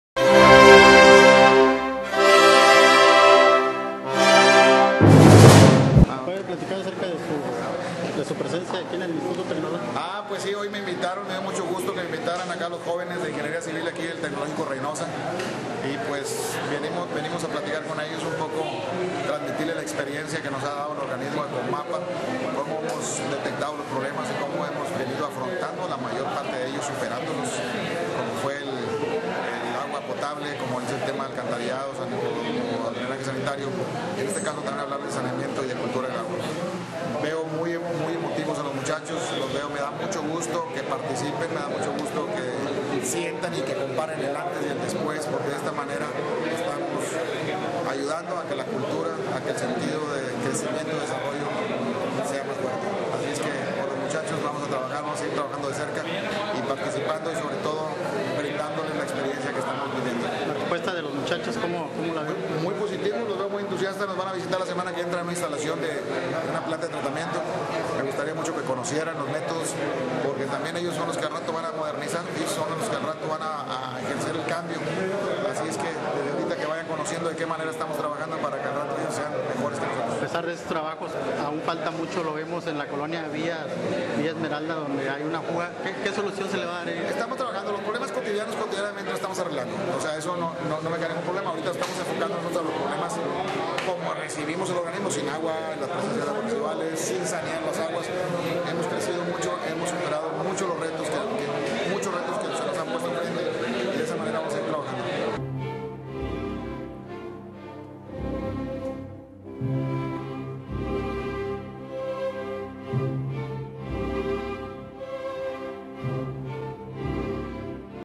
Esiquio Reséndez Cantú, Gerente de COMAPA Reynosa. Primer Foro de Ingeniería Civil ITR
Participación del gerente de COMAPA en el Primer Foro de Ingeniería Civil, realizado por el Instituto Tecnológico de Reynosa